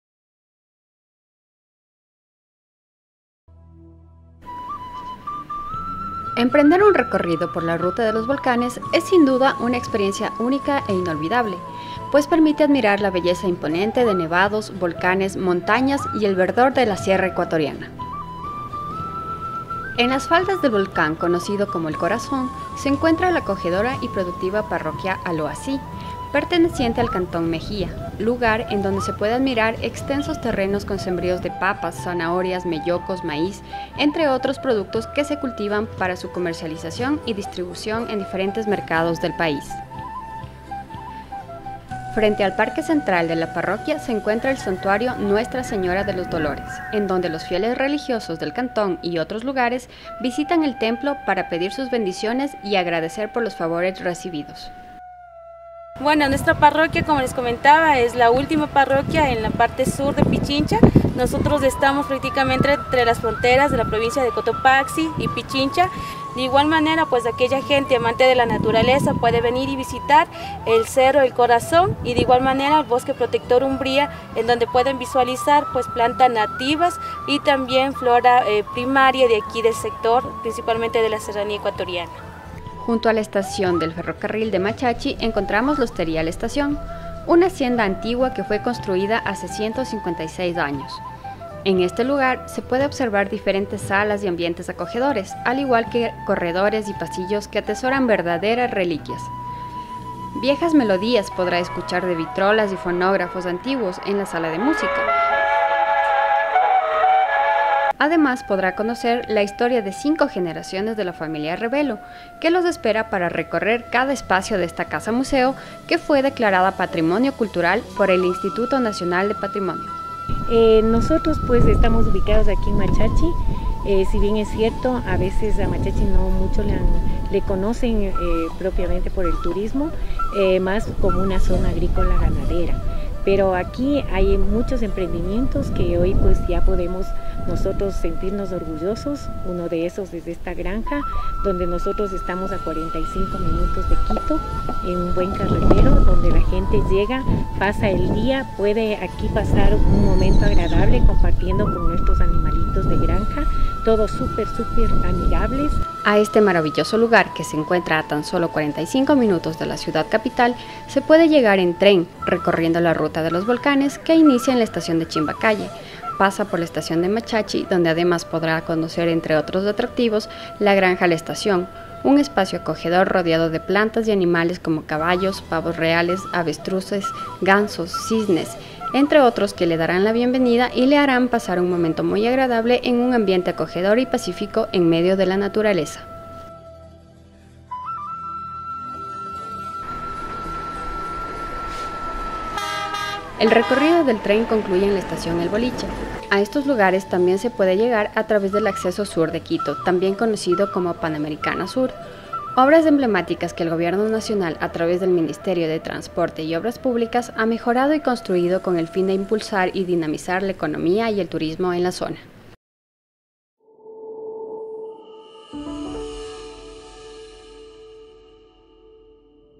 ALOASÍ Parroquia histórica, productiva y hospitalaria | Reportaje Especial
En las faldas  del  volcán  “El  Corazón”  se  encuentra  la  acogedora  y  productiva Aloasí, parroquia del cantón Mejía, en este lugar se puede observar grandes sembríos de papa, zanahoria, melloco, maíz, entre otros productos cultivados para su comercialización y distribución en diferentes mercados del país.